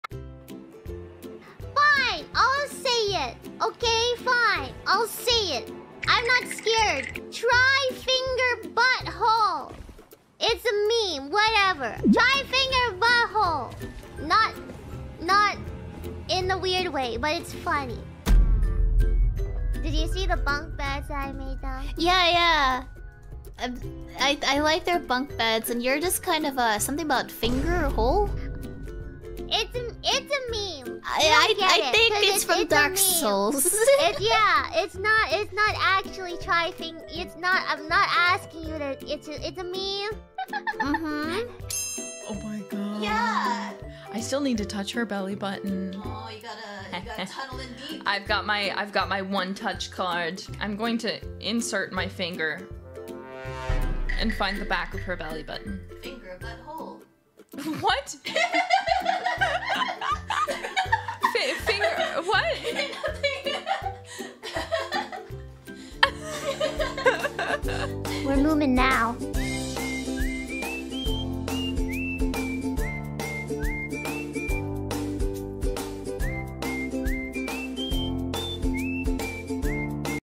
[ENG SUB/Hololive] Nerissa got caught off guard when Shiori used the ancient spell
Credits :
BGM